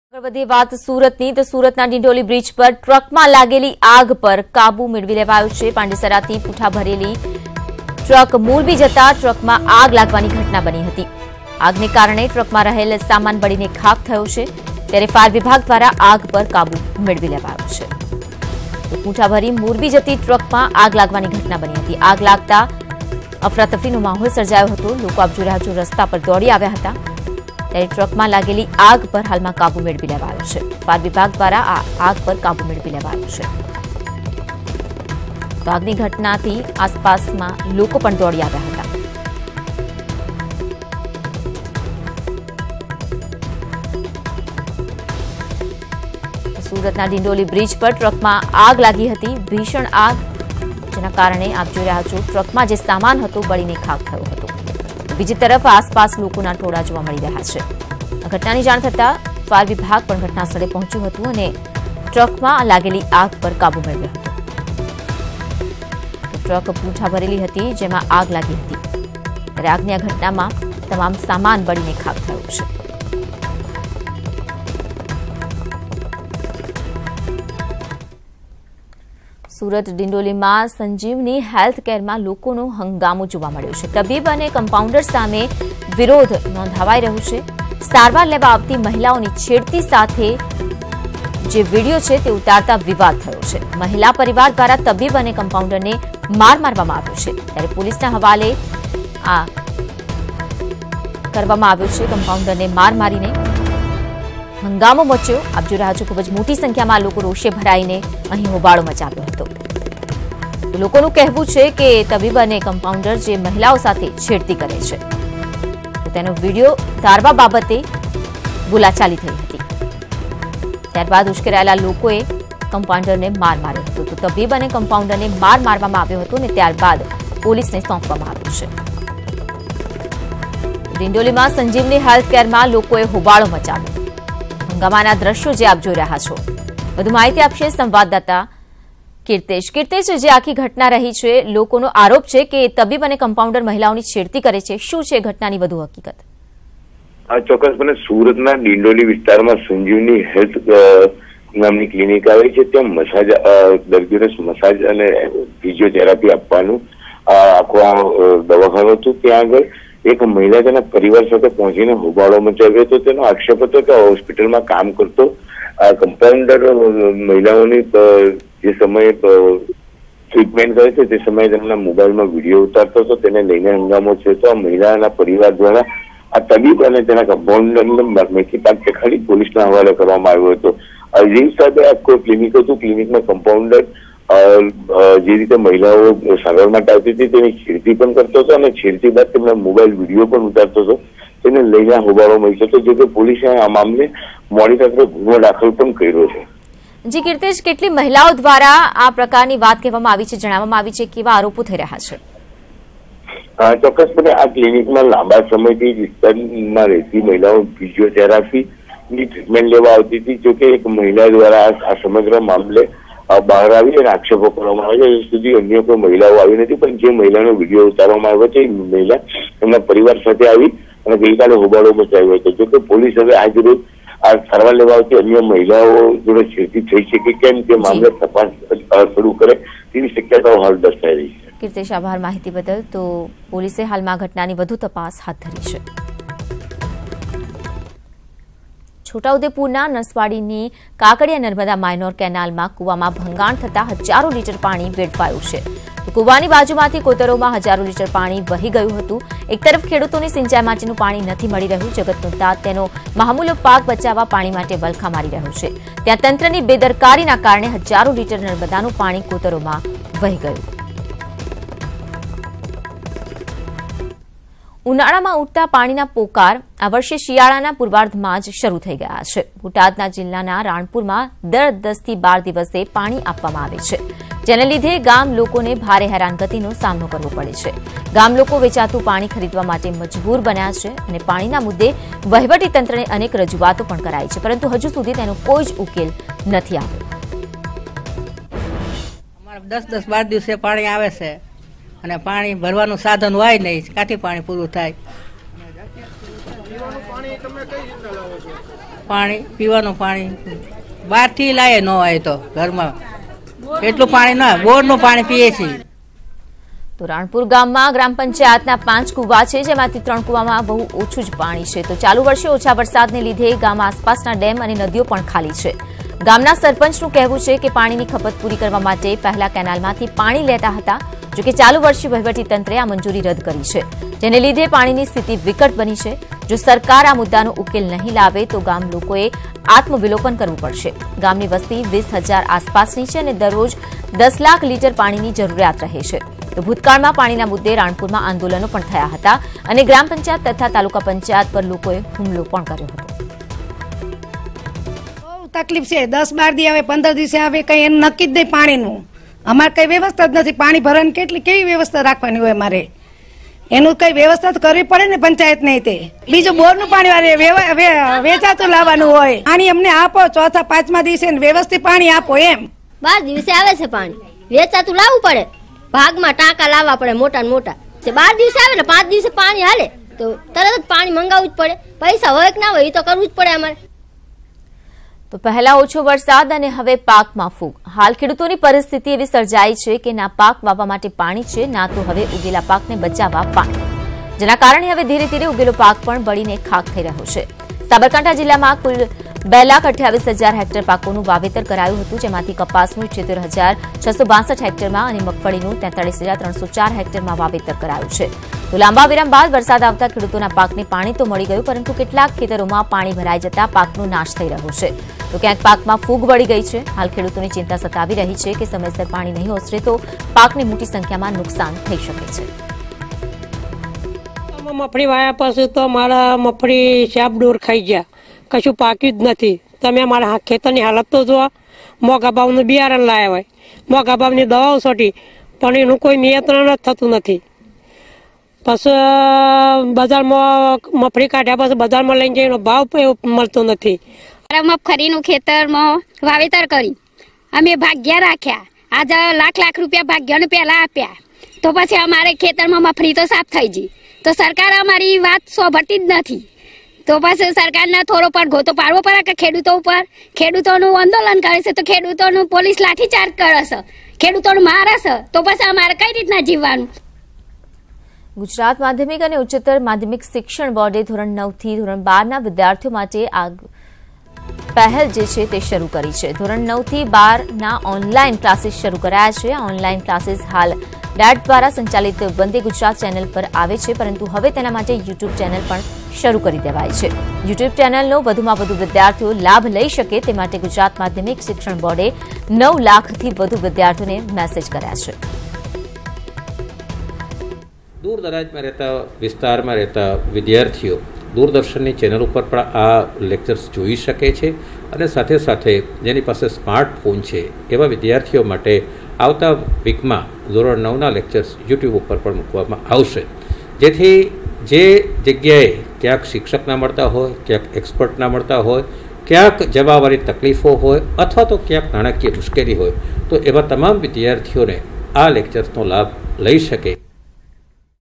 Surat : સંજીવની હેલ્થ કેરમાં લોકોનો હોબાળો | SAMACHAR SATAT | News18 Gujarati
ડિંડોલી વિસ્તારમાં આવેલ સંજીવની હેલ્થ કેરમાં લોકોએ હંગામો મચાવ્યો.કમ્પાઉન્ડર દ્વારા મહિલાઓની છેડતી થતી હોઈ લોકોએ માર માર્યો.